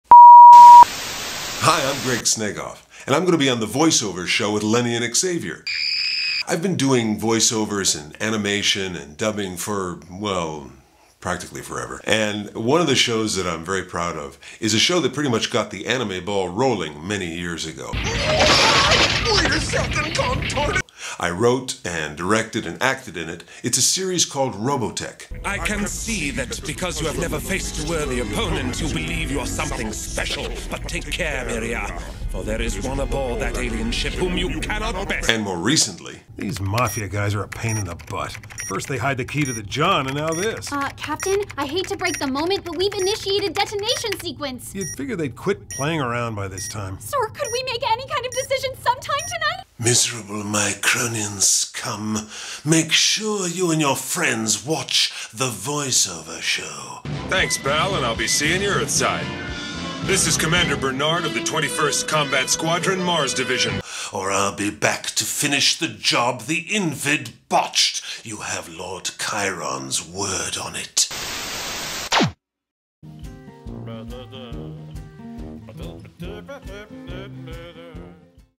It's Robotech Voice Actor, Writer and Director, Greg Snegoff!- Get Ready Anime Fans!
Special thanks to Anime legend and Voice Over talent Greg Snegoff for sending us this message!

Greg is the writer, director, and voice of character Scott Bernard in the classic 80's sci-fi anime series Robotech! We can't wait to have you on the show Greg!

+ TheVoiceOverShow.com

FOLLOW TVOS ON TWITTER HERE!

LIKE THE OFFICIAL TVOS FACEBOOK PAGE!